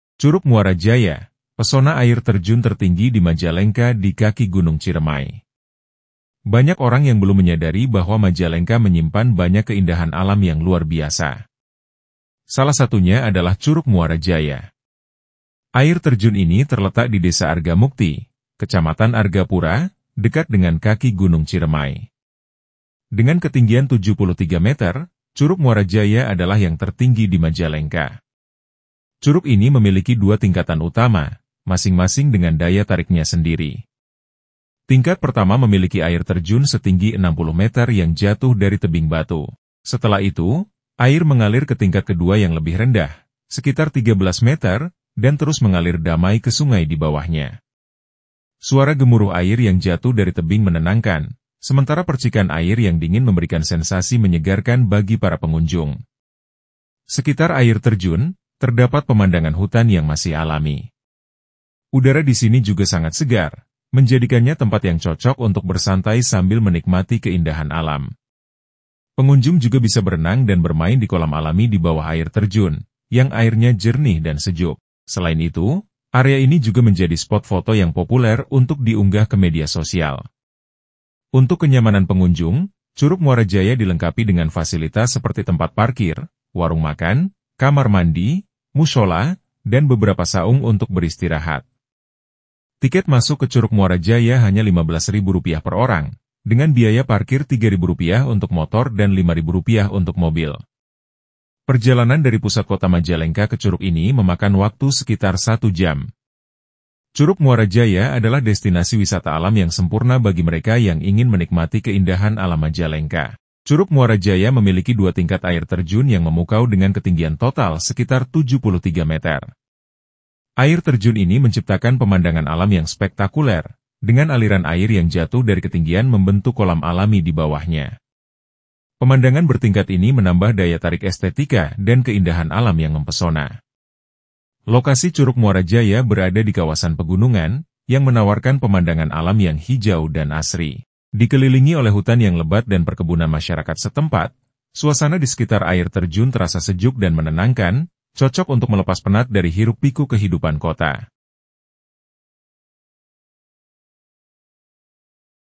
Curug Muara Jaya: Pesona Air Terjun Tertinggi di Majalengka di Kaki Gunung Ciremai
Banyak orang yang belum menyadari bahwa Majalengka menyimpan banyak keindahan alam yang luar biasa. Salah satunya adalah Curug Muara Jaya.
Air terjun ini terletak di Desa Argamukti, Kecamatan Argapura, dekat dengan kaki Gunung Ciremai. Dengan ketinggian 73 meter, Curug Muara Jaya adalah yang tertinggi di Majalengka.
Curug ini memiliki dua tingkatan utama, masing-masing dengan daya tariknya sendiri. Tingkat pertama memiliki air terjun setinggi 60 meter yang jatuh dari tebing batu.
Editor: Raden M Rizky
Ikuti WhatsApp Channel Sukabumiupdate.com untuk mengakses berita terkini dan terpercaya dari kami. Ketik di browser kamu sekarang juga!